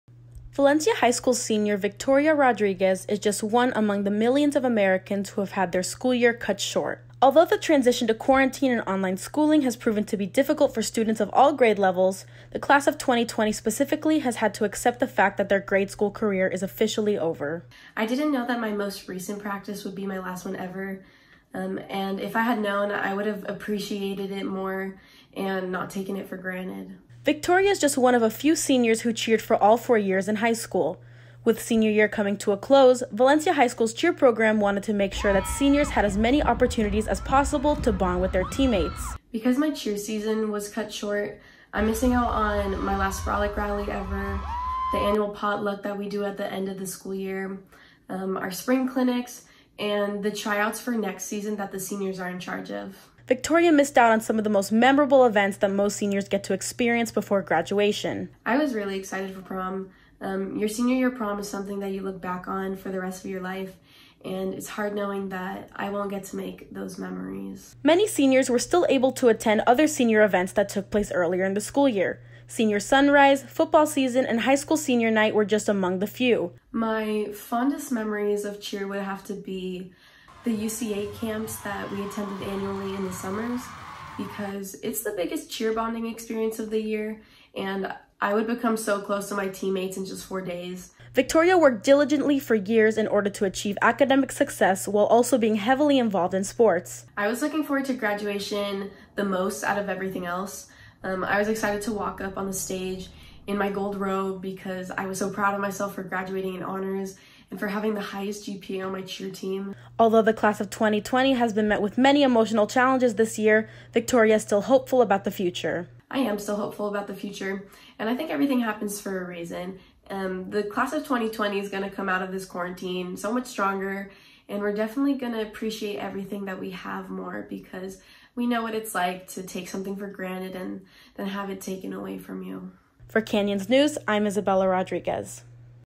Class of 2020 Senior - The End of a Season
Watch how Valencia High School senior Victoria Rodriguez reflects on her cheer and academic career and stays positive after the cancellation of her Prom and Graduation.